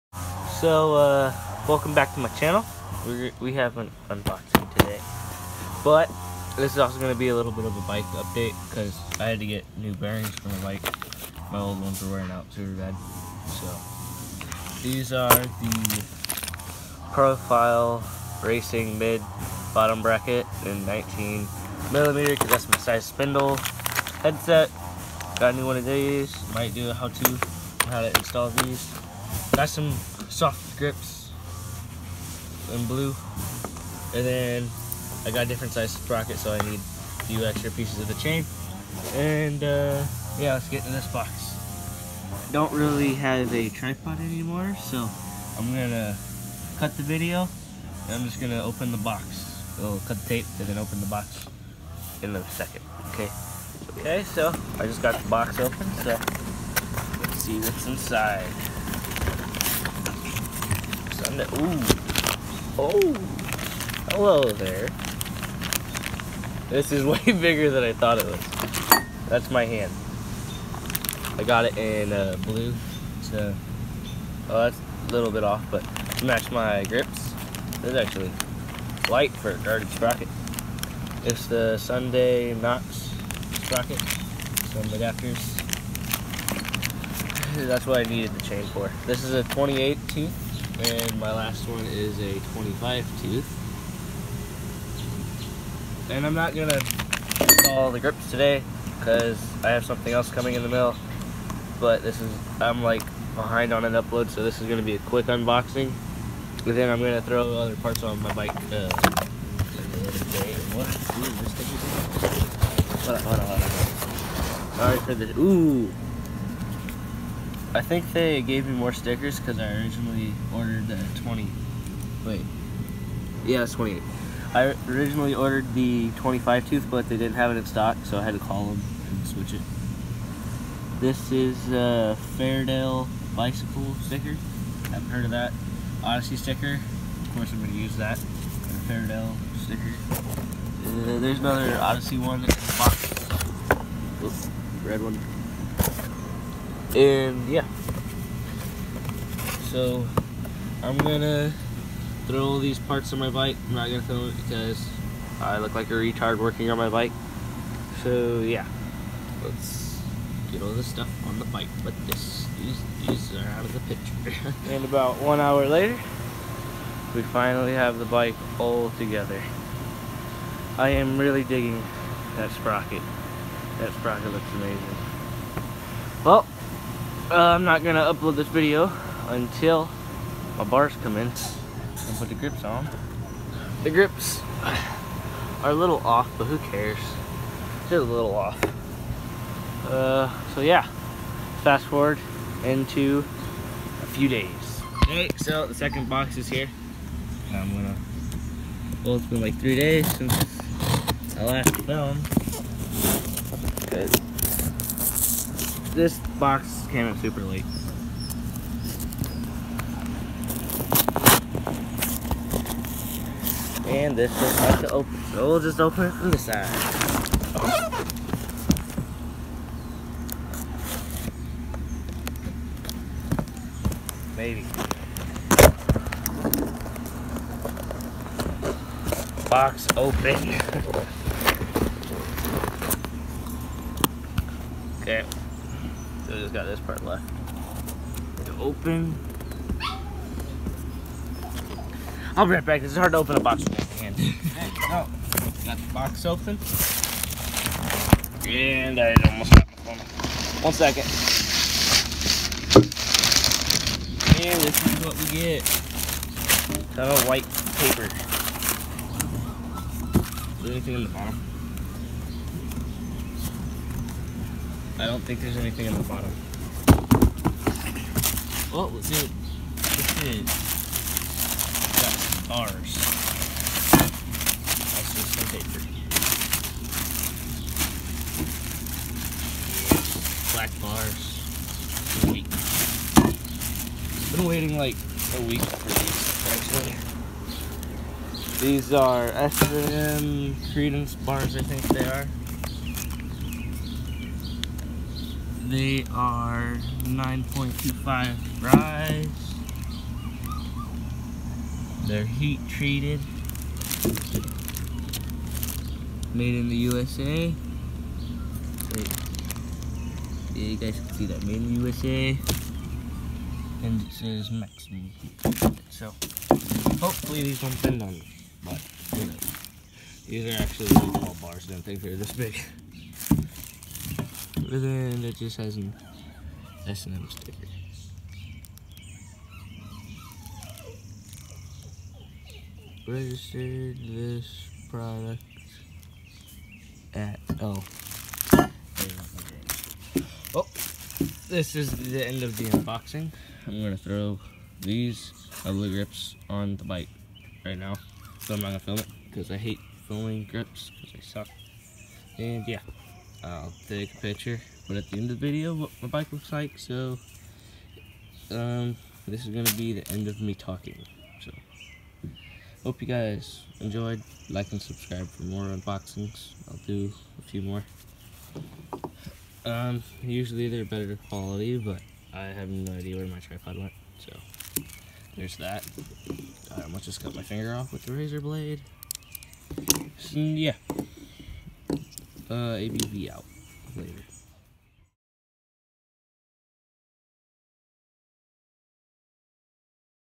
Albes Bmx Unboxing
Sorry the video is sideways. I need a new camera/phone
Riding videos in the next video hopefully
Sorry I was a day late it was my sisters b day and I didn’t get to film :/
ABV Out